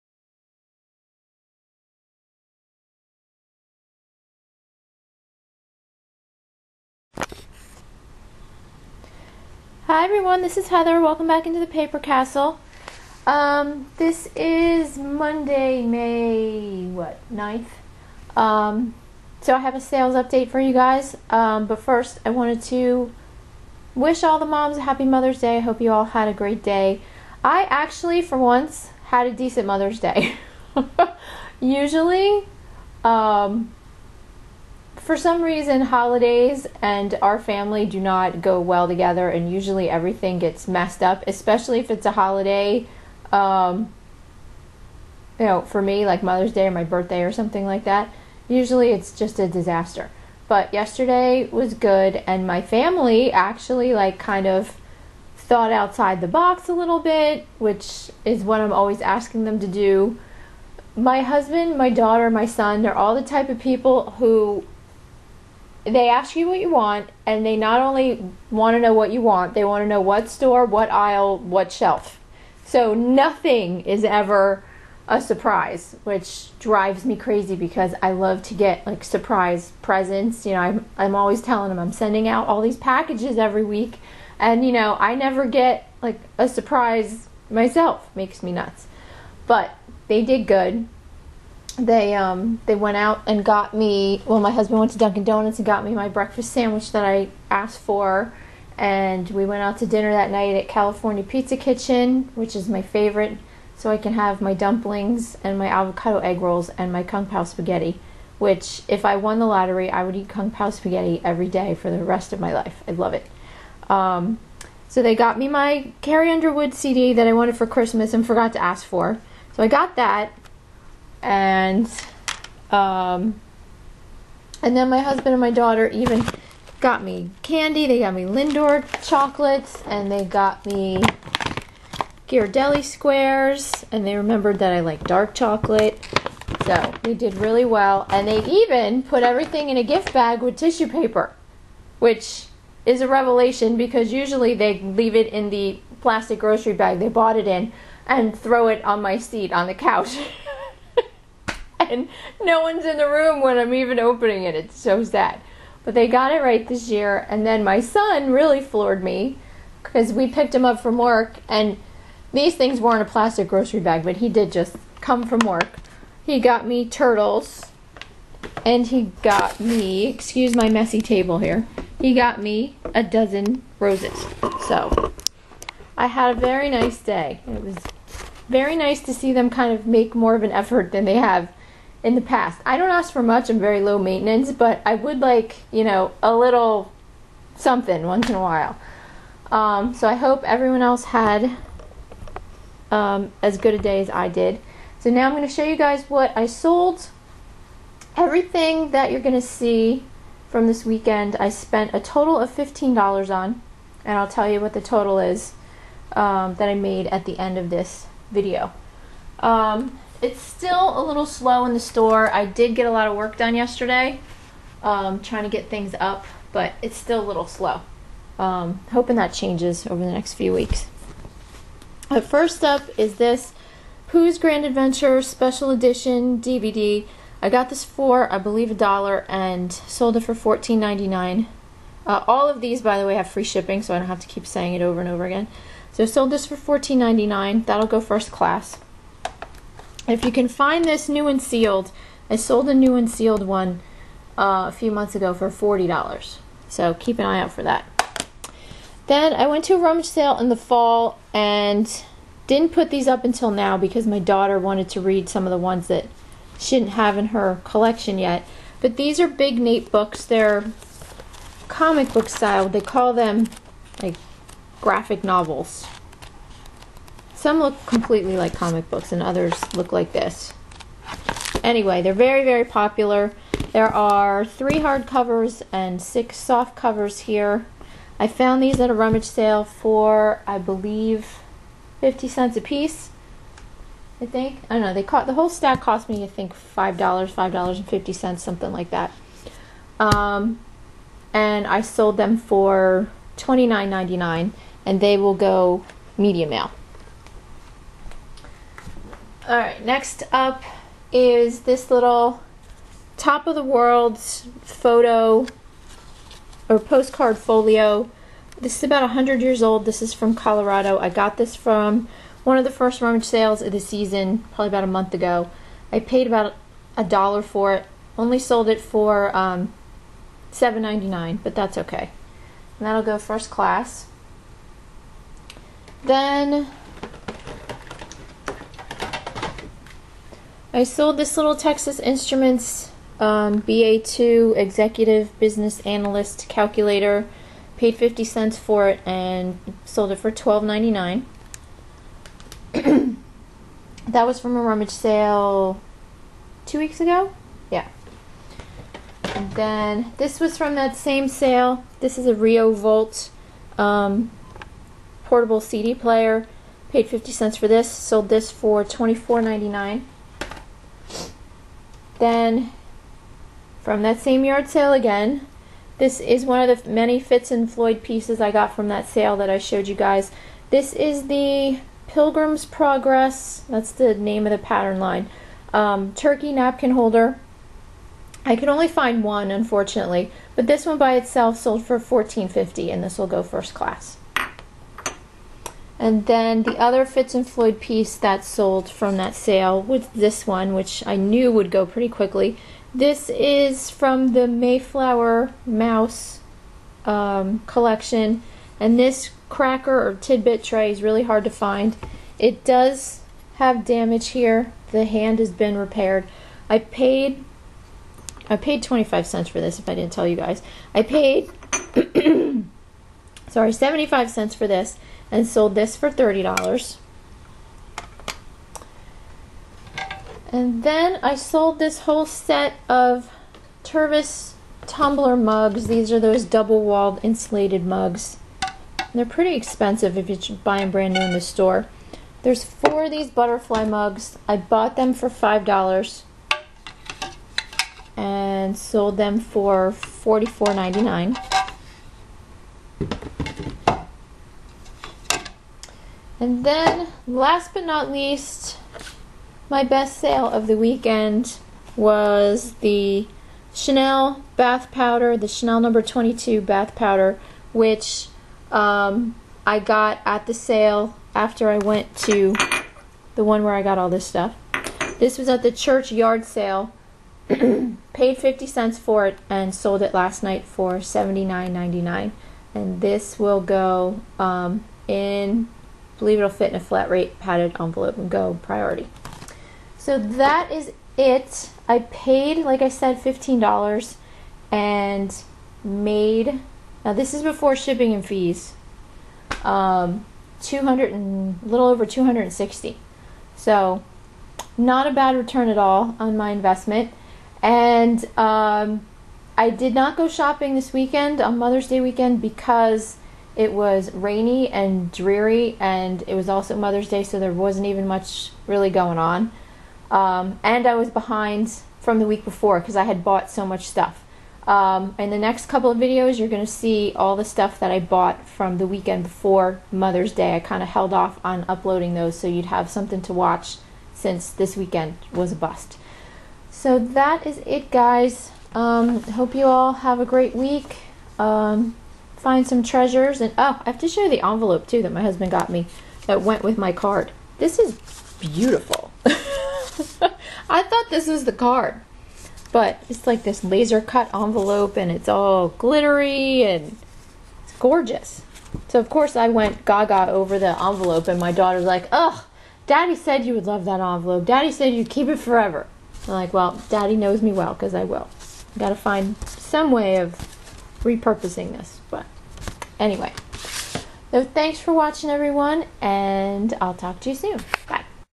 Ebay Sales Update & Happy Mother's Day Wishes - May 9, 2016
Here's a sales update from Mother's Day weekend and I share a little about how my Mother's Day went.  My family did a really good job!  I hope all the moms out there had a wonderful day!